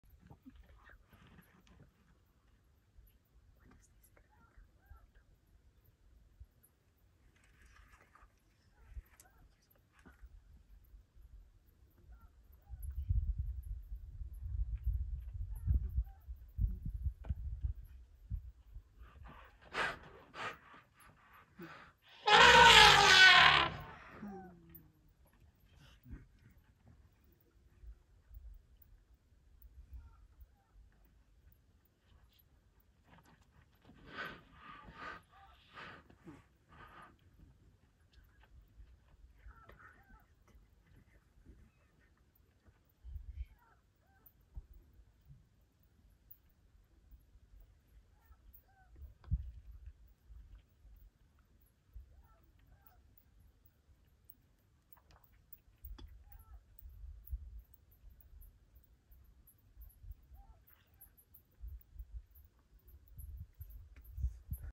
Elephant Chases Rhino
Adolescent elephant seeking out black rhino to chase away at Okaukuejo waterhole, Etosha National Park, Namibia